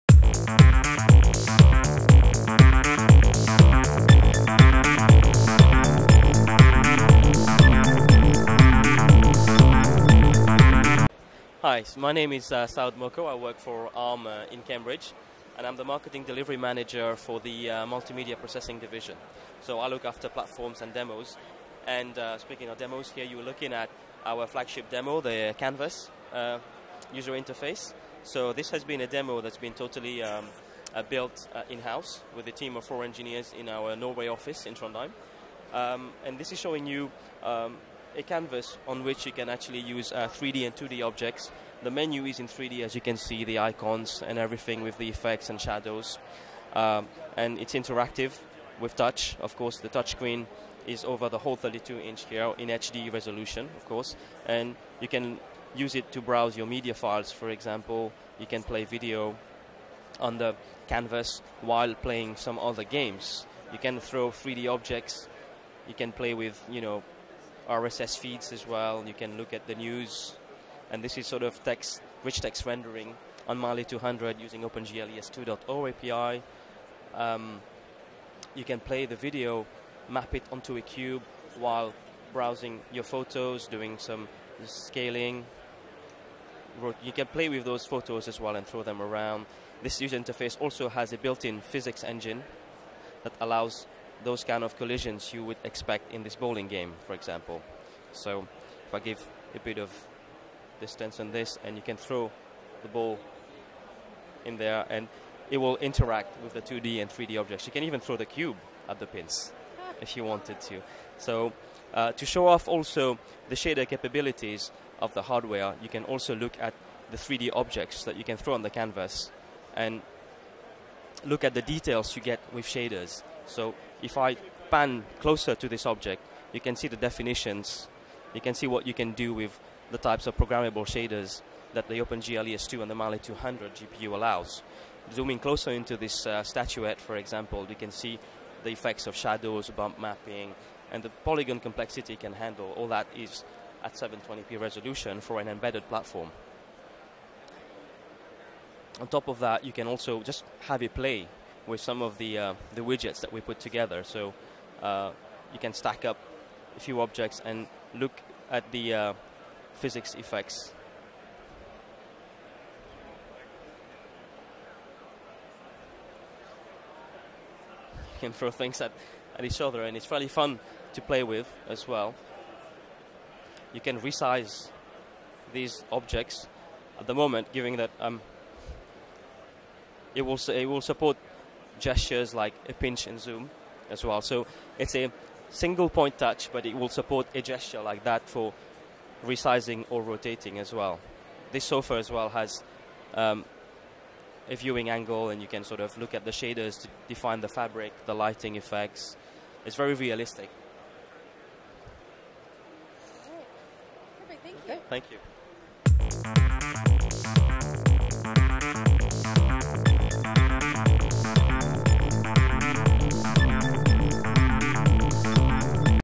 GDC 2010: Mali 2D & 3D + Multitasking 2 videos on a big ...
Walk through of an in house demo of 'Canvas' which shows off 2D & 3D objects and games while running multiple videos.  This is all done on a big screen so its a nice indication of the power of this processor that we have yet to see roll out into the mainstream apart from a few products that we can only find in China.